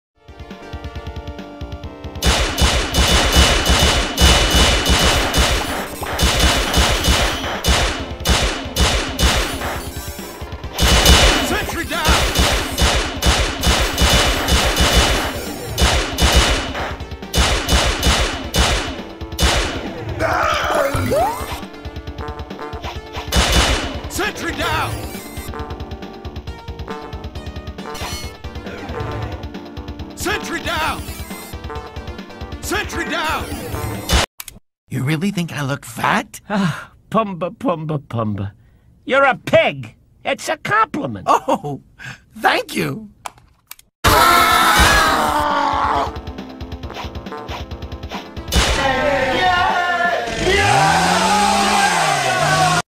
Woodpecker and Crow interrupt 4 Video Game Collection
The 2 Players are ready to pop some ballons and avoid some enemies Crow pauses and called he was fat WARNING DO NOT SET THIS VIDEO TO MADE IT FOR KIDS!